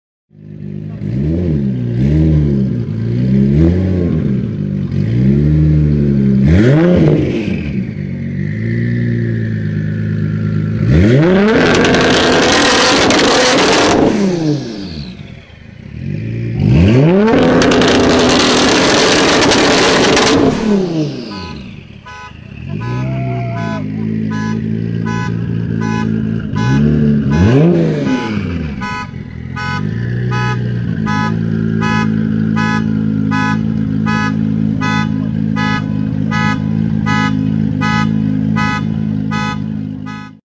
1750HP Nissan GT-R looks and sounds so brutal 2018
Nissan GT-R looks and sounds so brutal 2018
If you dislike, just leave your comment so i will make it better!!